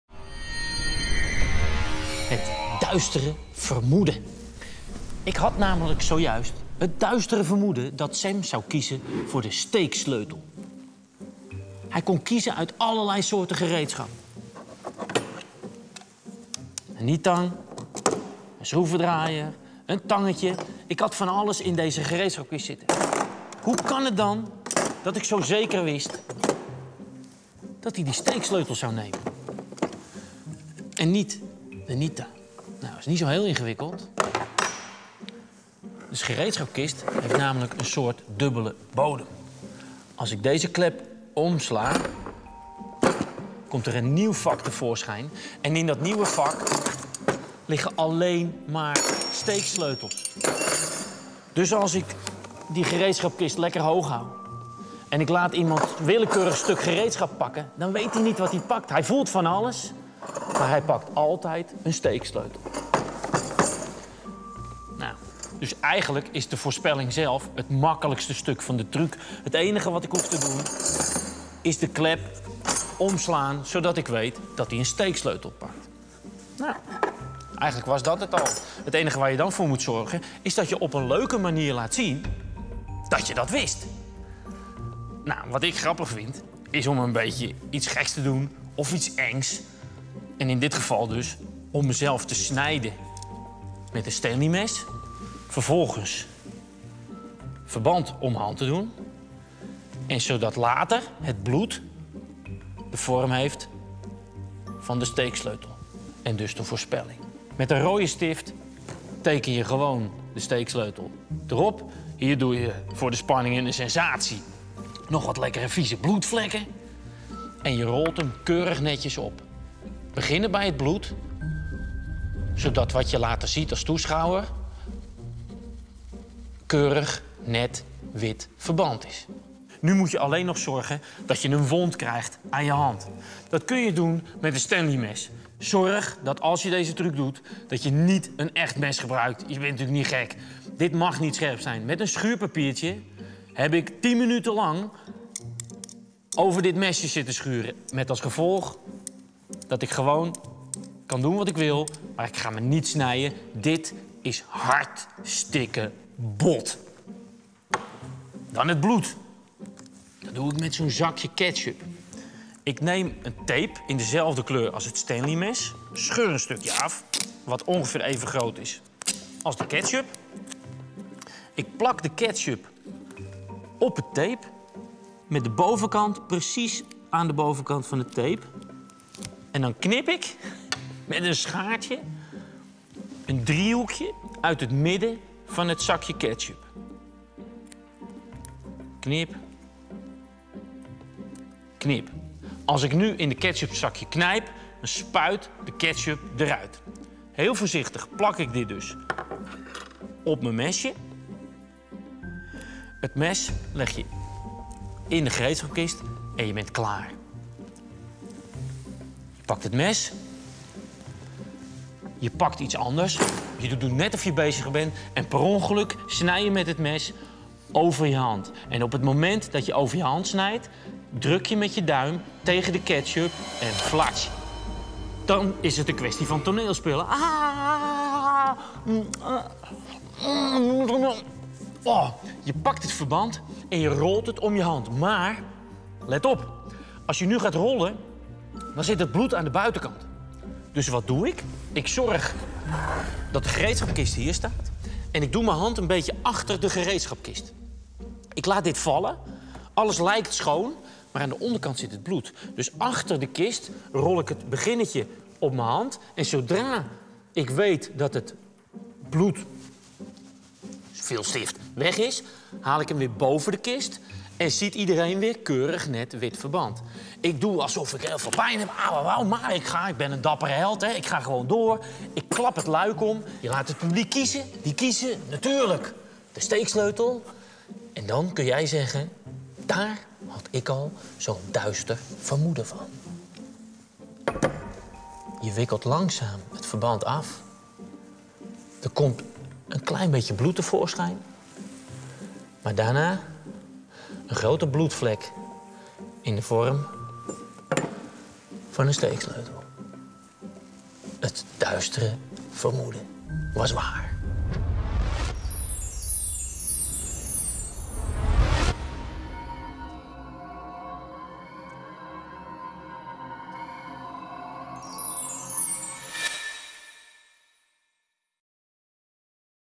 De Magische Winkel - Uitleg truc De Metamorfose
Het lijkt zo simpel, maar ja hoe werkt de truc De Metamorfose? Bekijk de uitleg van Hilbert Geerling.